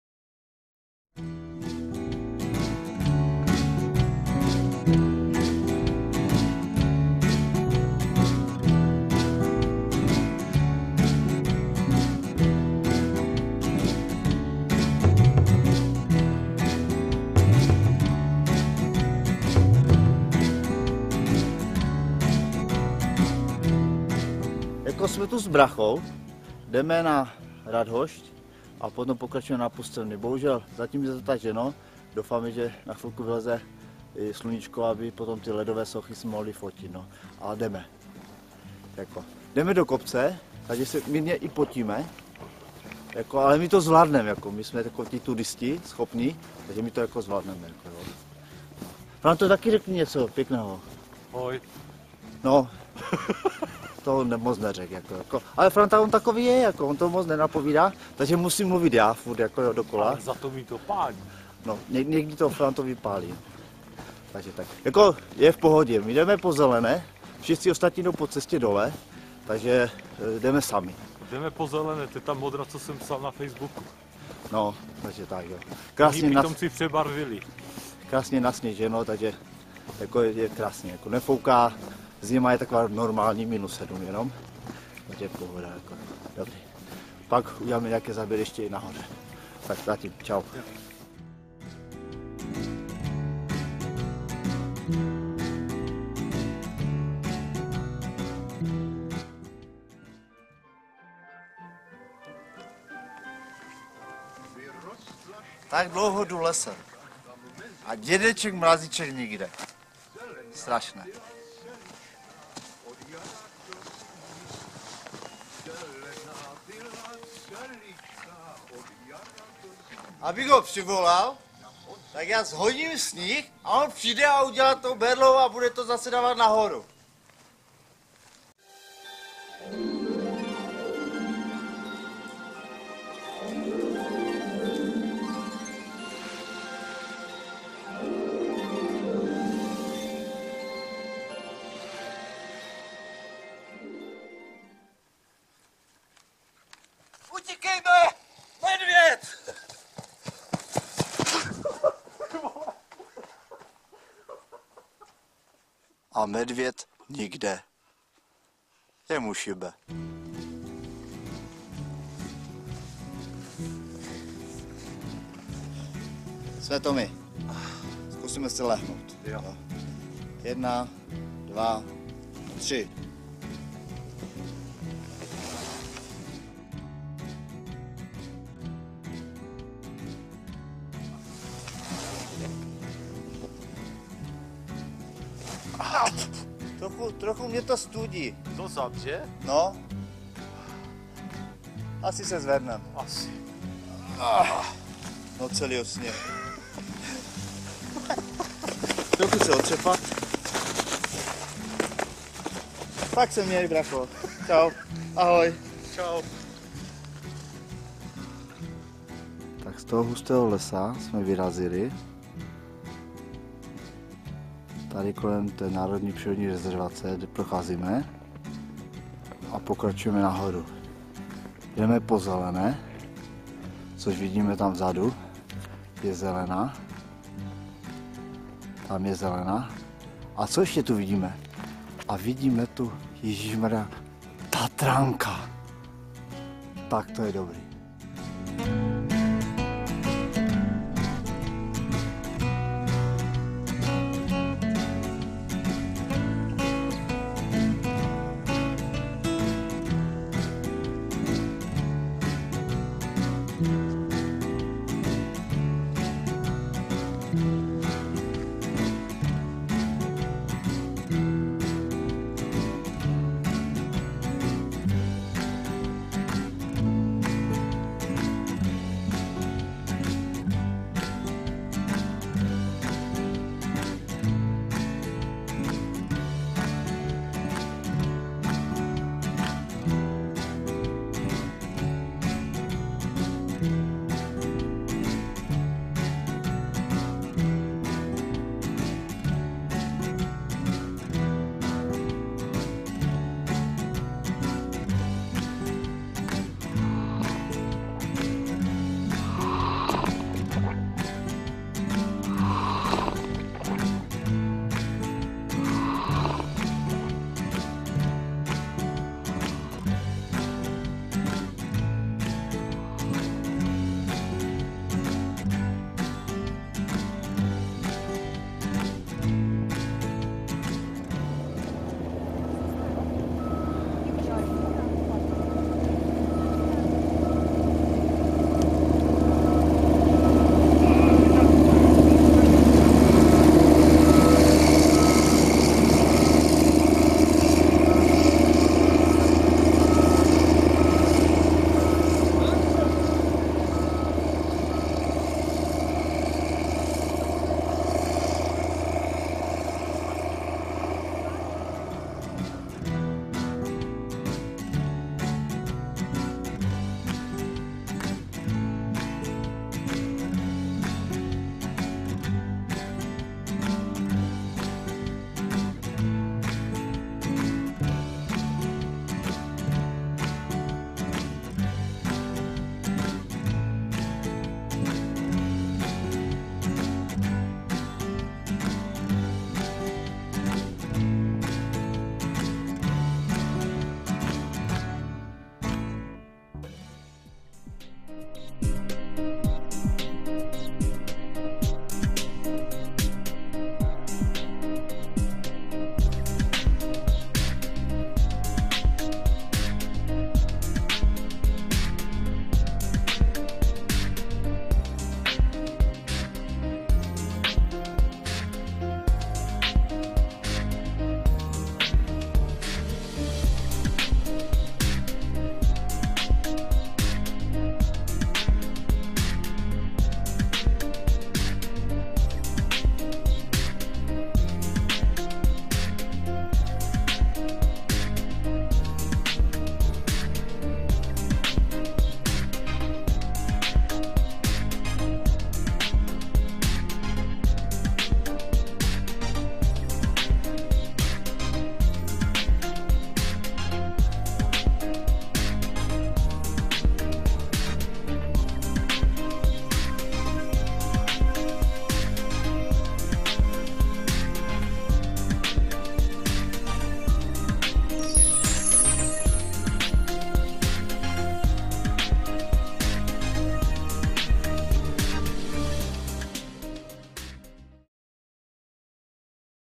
Zimní Beskydy 2013
s bráchou na trase Ráztoka - Radhošť - Radegast - Pustevny